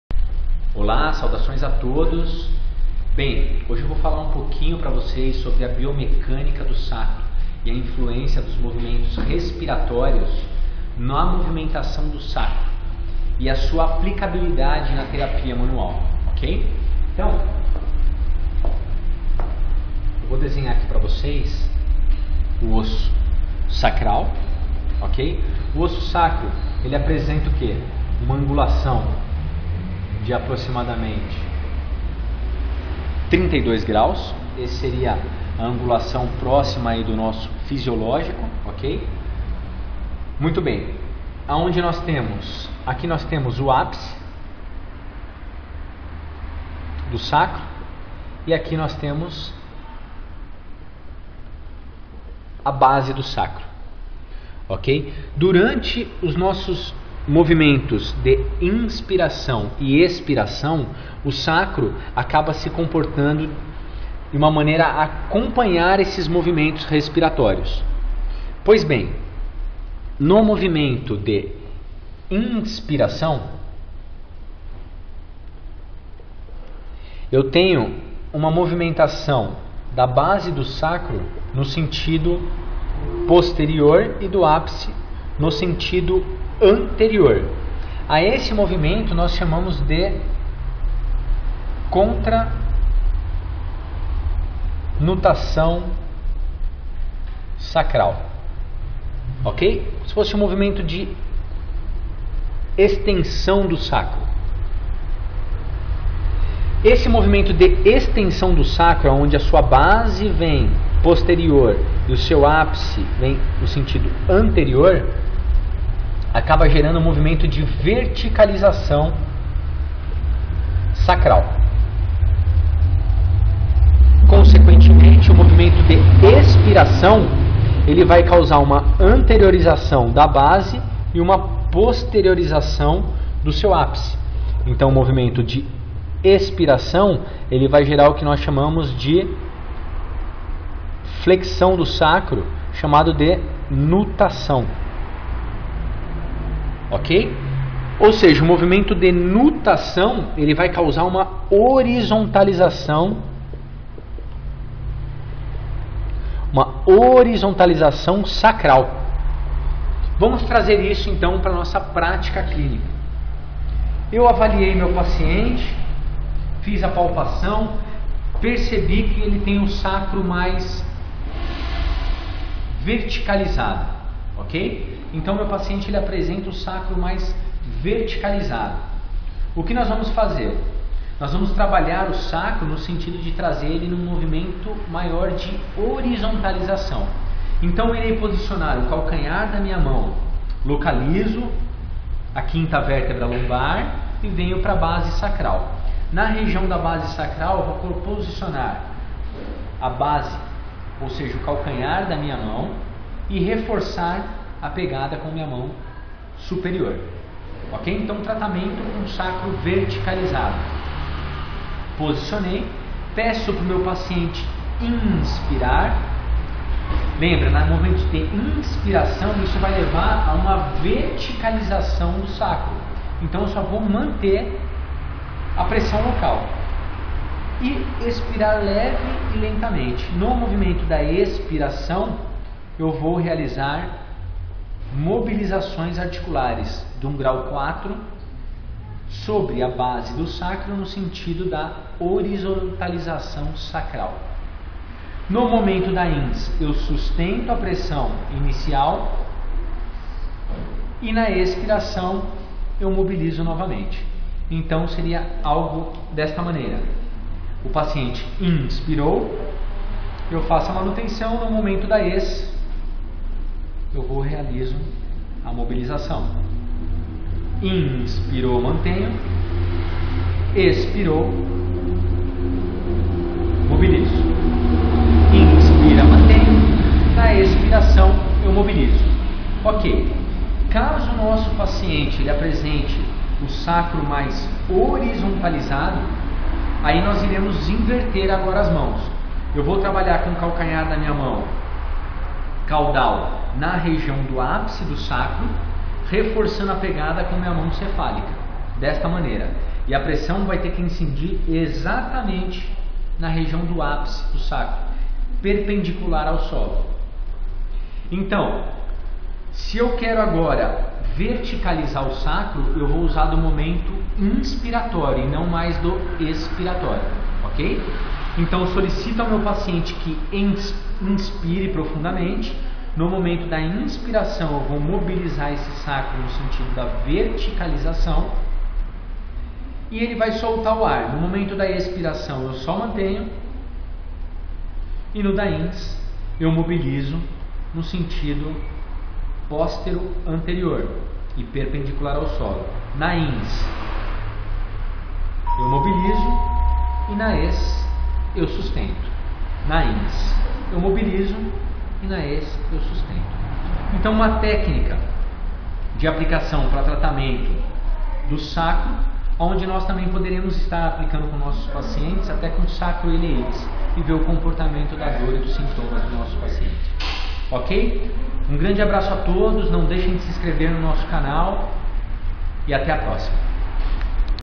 Tratamento do Sacro com Terapia Manual
Neste vídeo o fisioterapeuta, Dr. Robson Sitta, realiza uma orientação para o tratamento com a terapia manual no sacro